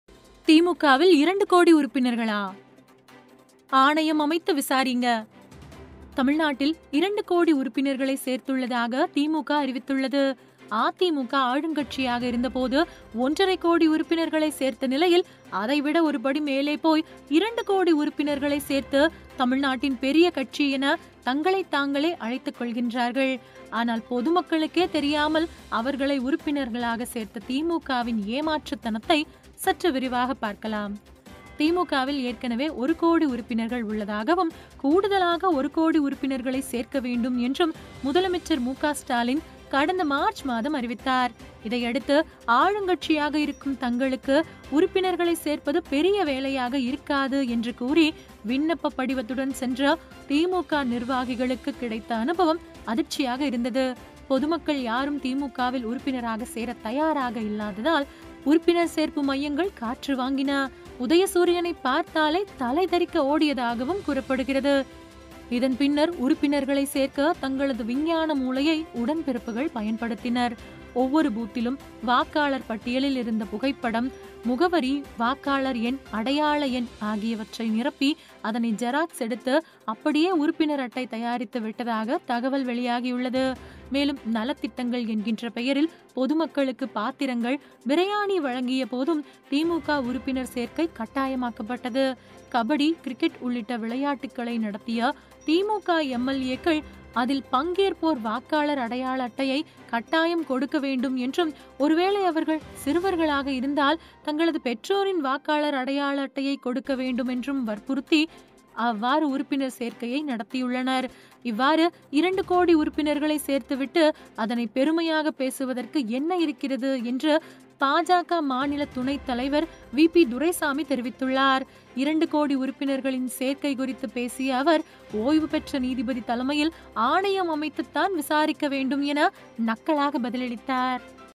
ஒரு கோடி உறுப்பினர் சேர்க்கையா;கிண்டலடிக்கும் வி.பி துரைசாமி
/ oreynaadu